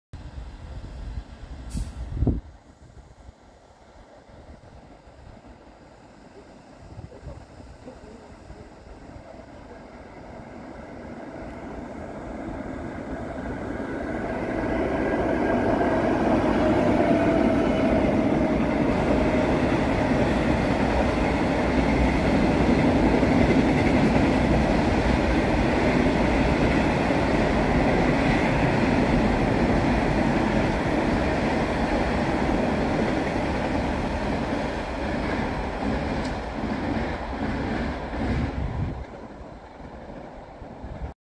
4S49 - DB90s - 90039 & 90035 on the late Blue box movement 💙 to Grangemouth 19 minutes early
On a day when the DB90s were once again rotated, with Bumblebee3 (90020) and Christine (90037) undestood to have been rotated out for a fresh pair of DB90s - possibly Donald (90021) and Bill (90028).